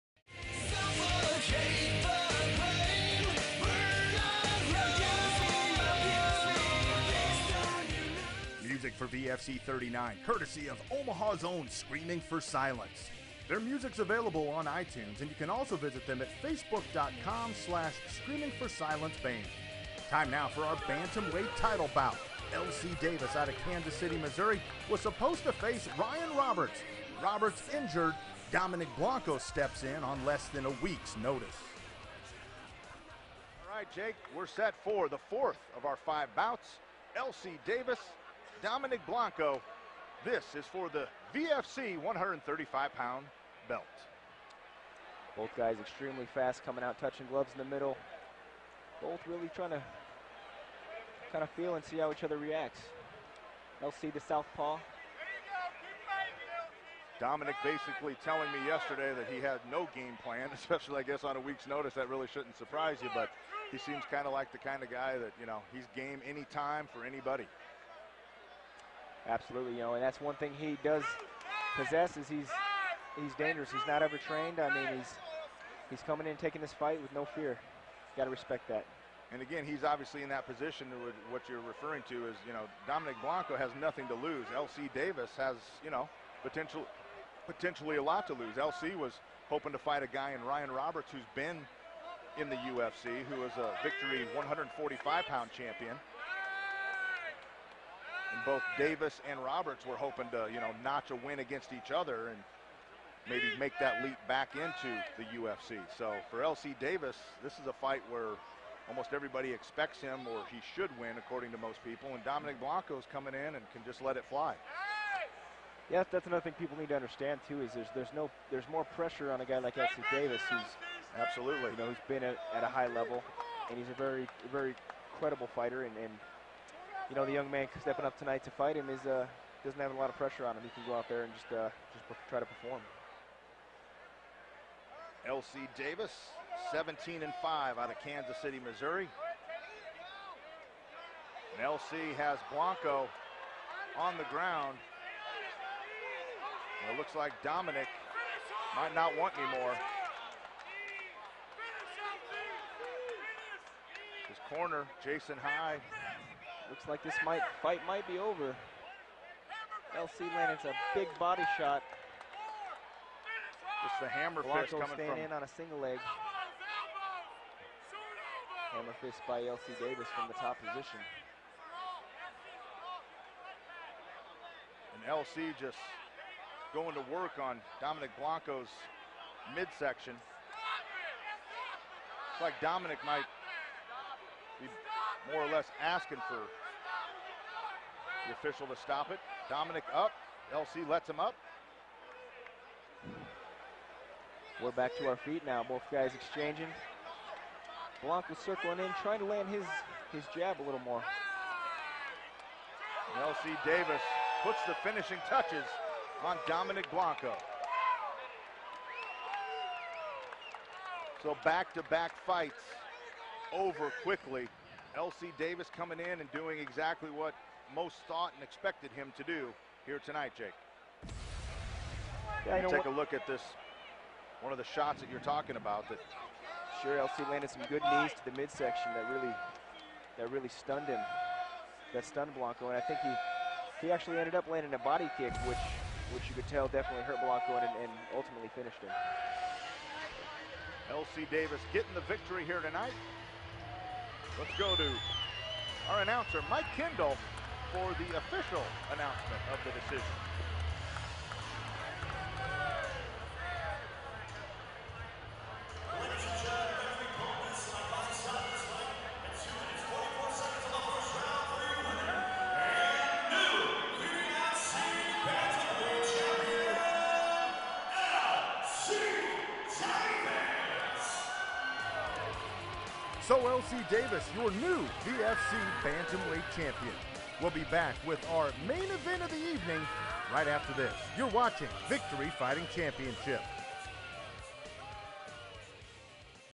VFC on FOX - VFC 39 BROADCAST - FIGHT #4 - DAVIS vs BLANCO
March 30, 2013 - VFC 39 - LC Davis vs Dominic Blanco. Matt Kelly calls the action & UFC Top-5 Welterweight Jake Ellenberger provides the color commentary.